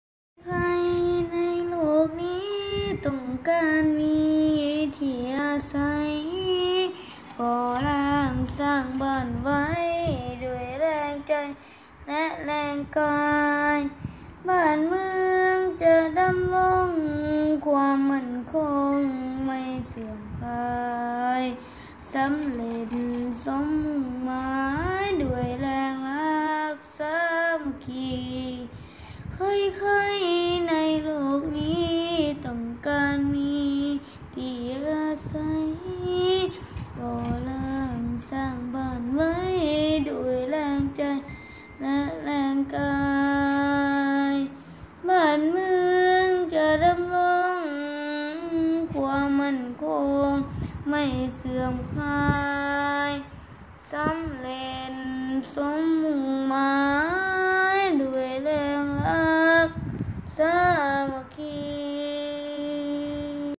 เด็กชายไตรภพ สมใจ ชั้น ป.5โรงเรียนวัดพระเชตุพนวิมลมังคลาราม
อ่านทำนองเสนาะ เรื่องบ้านของใคร